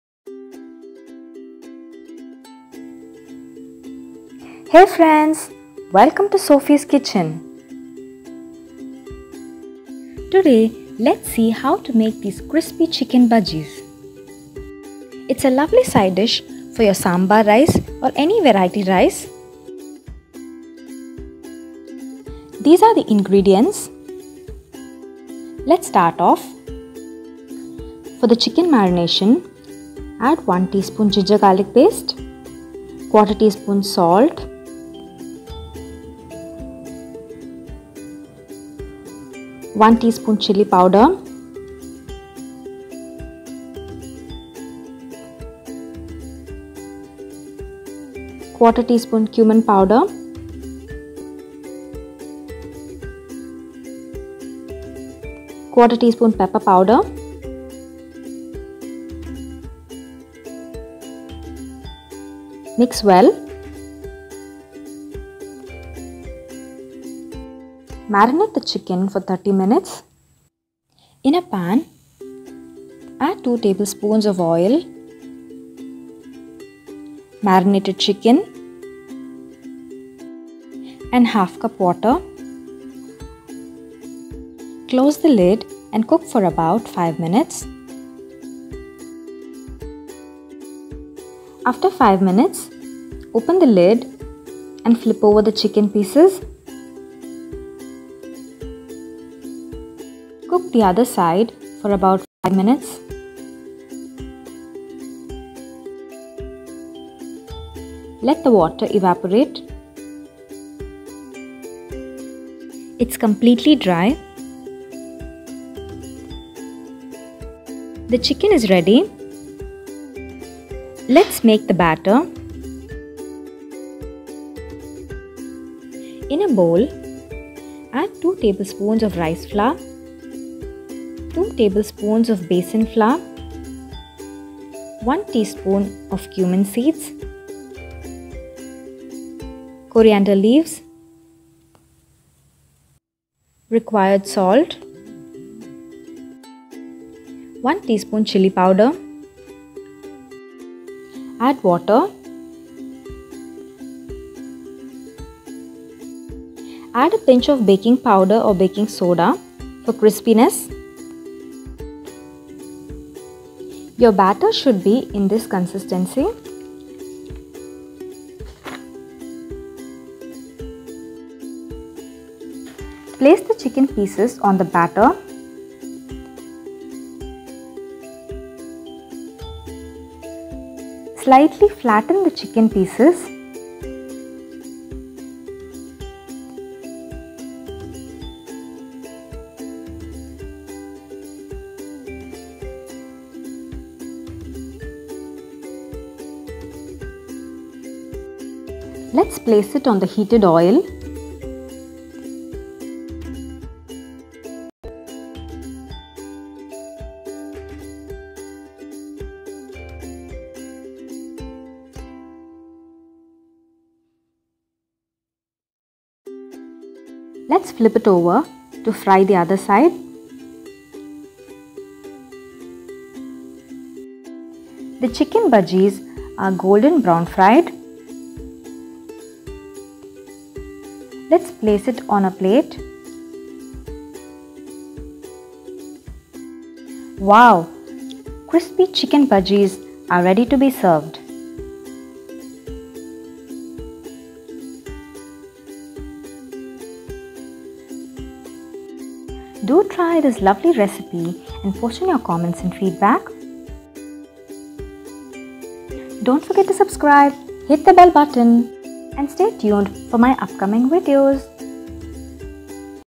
Chicken Bajji | Starters | Fritters | Chicken dish | Snacks | Sophie's kitchen
Crispy Chicken Bajji:

This is a lovely side dish for sambar rice or kurma or any curry rice or any variety rice.

It’s also a great combo for fried rice and noodles too.

It’s a great snack or starter too

Ingredient’s:

Gg paste 1 tsp
Cumin powde ¼ tsp
Pepper ¼ tsp
Rice powder 2 tbsp
Besan powder 2 tbsp
Required salt
1 tsp chilly powder
Coriander leaves
Cumin seeds 1 tsp
I pinch of baking powder ( optional)

Method:

For the chicken marination,  in a bowl, add Chicken 500 gms, gg paste 1 tsp, salt ¼ tsp, chilly powder 1 tsp, cumin powder ¼ tsp and pepper ¼ tsp.

In a pan, add 2 tbsp of oil, marinated chicken and ½ cup water. Close the lid and cook for about 5 min on each side.
Let the water evaporate and the chicken is ready.

For the bajji batter,  in a bowl add 2 tbsp of rice flour,  2 tbsp of besan flour, 1 tsp cumin seeds. 1 tsp chilly powder, required salt and 1 pinch of baking powder or baking soda.

Dip the chicken in the batter and place it on the heated oil. Fry it it its golden brown on both sides.

Your Crispy Chicken Bajjis are ready to be served.

Enjoy this lovely and tasty bajjis with your loved ones and do post in your comments.